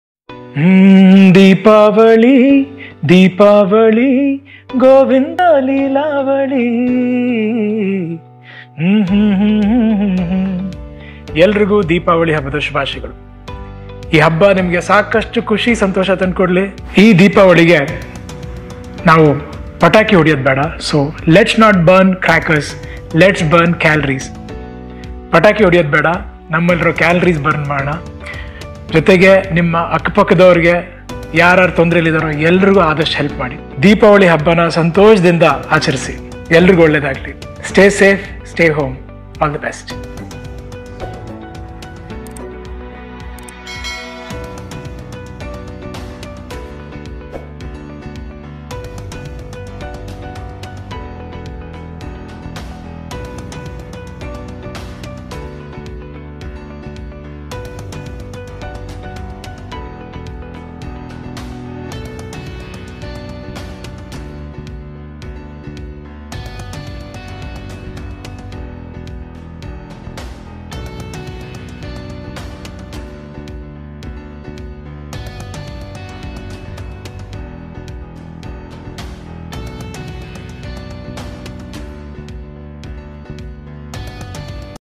😭ಪುನೀತ್ ರಾಜಕುಮಾರ್ ದೀಪಾವಳಿ ಶುಭಾಶಯ ಕೋರಿದ್ದ ಕ್ಷಣಗಳು..!!!
ಪುನೀತ್ ರಾಜಕುಮಾರ್ ನಮ್ಮನ್ನೆಲ್ಲ ಅಗಲಿ ಹೋಗಿದ್ದಾರೆ ಆದರೂ ಅವರ ಬಿಟ್ಟುಹೋದ ನೆನಪುಗಳು ಯಾವತ್ತಿಗೂ ನಮ್ಮ ಜೊತೆಗೆ ಇರುತ್ತವೆ ದೀಪಾವಳಿ ಶುಭಾಶಯ ಕೋರಿದ್ದ ಕ್ಷಣಗಳು